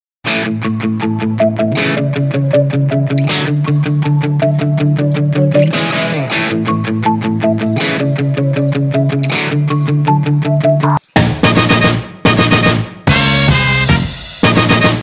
See lions come so close to humans near Gir forest Gujarat//સિંહો મનુષ્યની સાવ નજીક આવે
See lions come so close to humans // સિંહો મનુષ્યની સાવ નજીક આવી જાય એ છે ગુજરાતનું ગીર //